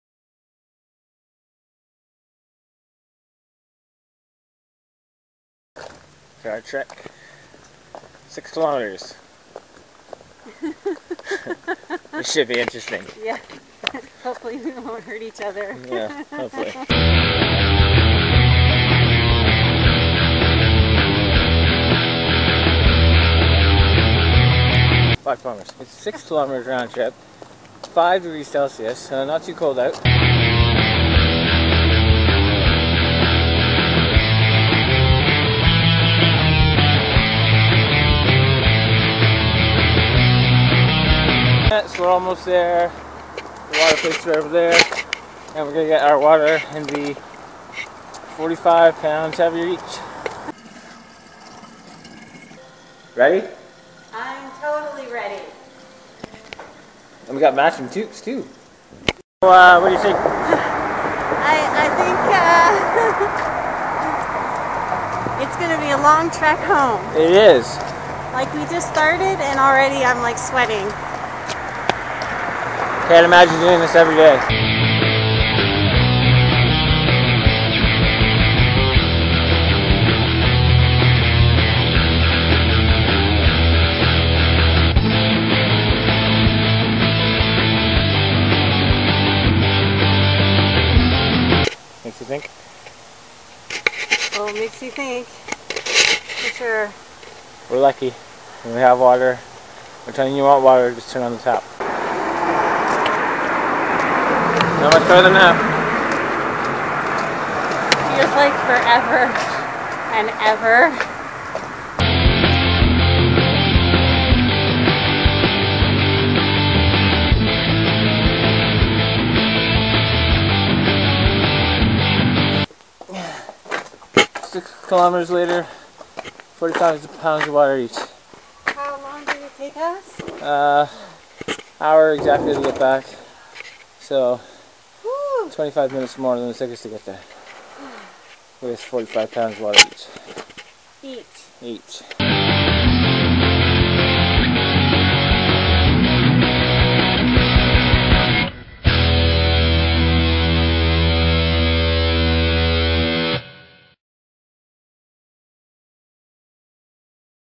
waterwalk - 6km walk for water
March 24th at 5PM we set out to walk 6km roundtrip in Cornwall Ontario Canada to pick up 19L of water.Millions of people on our planet walk for water every day.